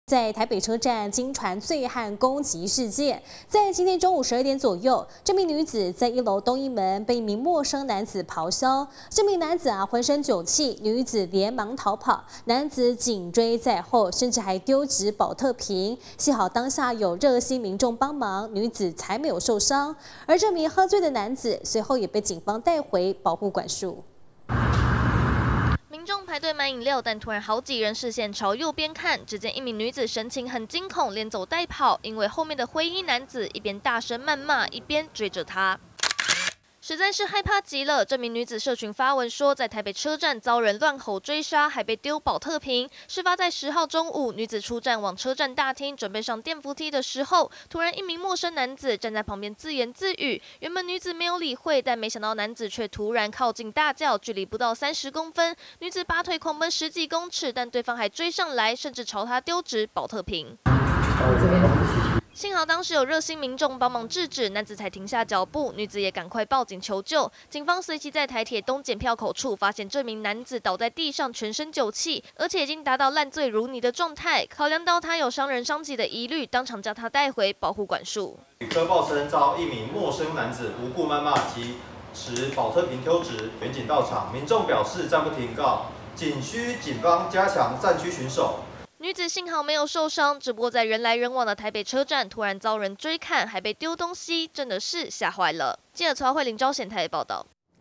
北車驚魂！ 女子遭陌生醉漢追打丟寶特瓶
台北車站驚傳醉漢攻擊事件！今天(10日)中午12點左右，一名女子在1樓東一門，遭陌生男子咆哮，對方渾身酒氣，女子嚇到連忙逃跑，但男子仍持續追趕，甚至丟寶特瓶狠砸，幸好當時有熱心民眾幫忙，女子才沒有受傷，而這名醉漢，隨後被警方帶回保護管束。